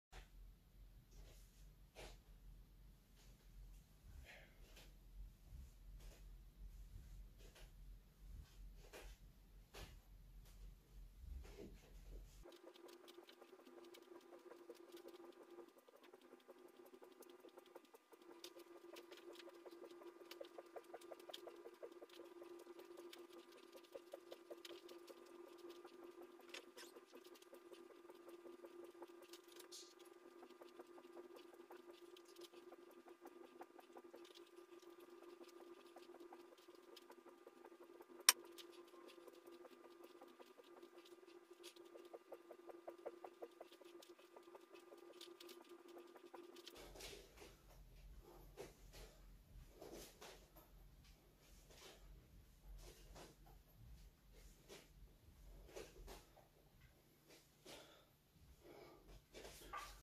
200 suburi challenge
How do you get into the zone? Challenging yourself until you have nothing left can create a state of nothingness inside you. Then there is nothing external that matters, only observing your own breath, body, and the present task.
Here, at home, I do my 200 daily cut challenge using the heavy bokken (suburito or suburi bokken).